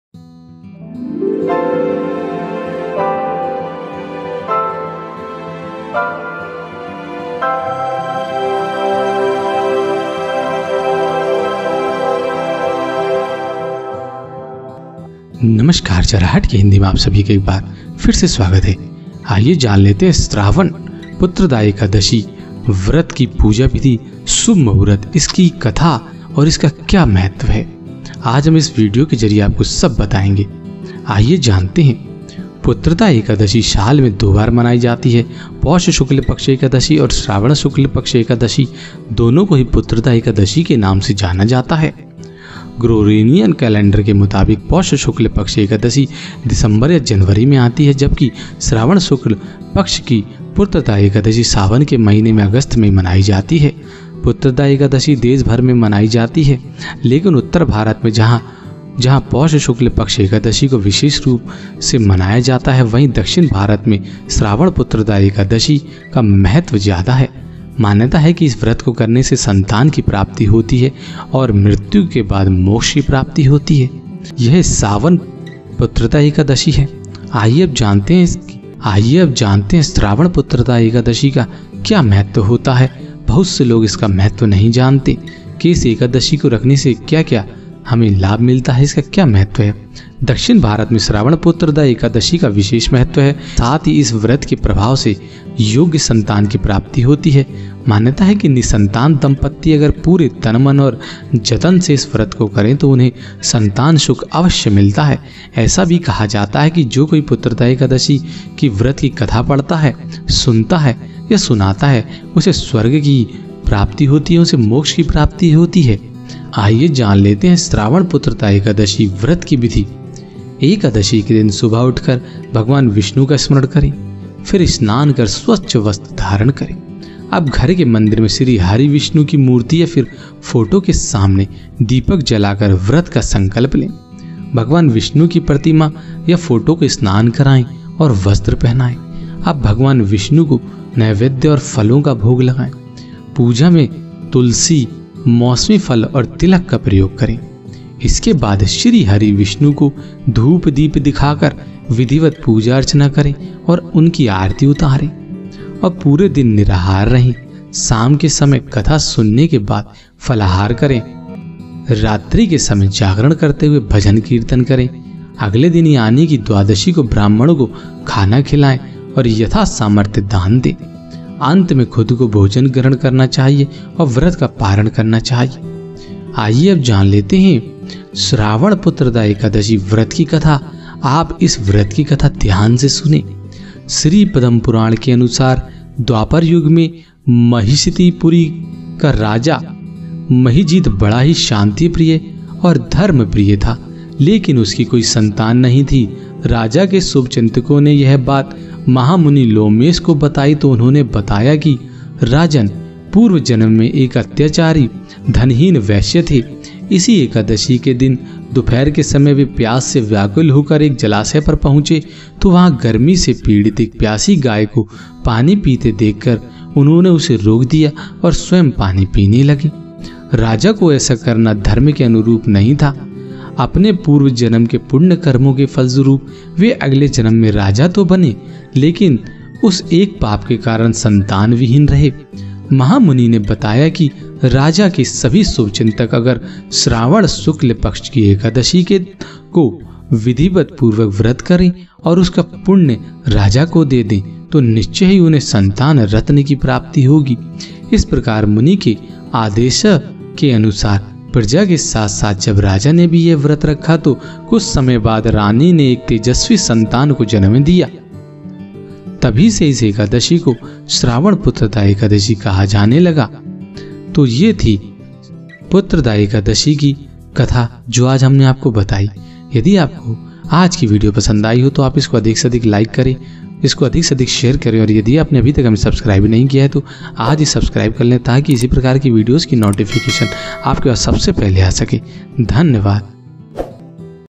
Shravana putrada Ekadashi 2021: श्रावण पुत्रदा एकादशी पूजा विधि और संपूर्ण कथा
श्रावण पुत्रदा एकादशी तिथि
श्रावण पुत्रदा एकादशी 18 अगस्त 2021, बुधवार को है.

श्रावण पुत्रदा एकादशी शुभ मुहूर्त
एकादशी तिथि प्रारम्भ- 18 अगस्त 2021 को 03:20 एएम
एकादशी तिथि समाप्त- 19 अगस्त 2021 को 01:05 एएम

चाणक्य नीति पुस्तक खरीदने के लिए नीचे क्लिक करें :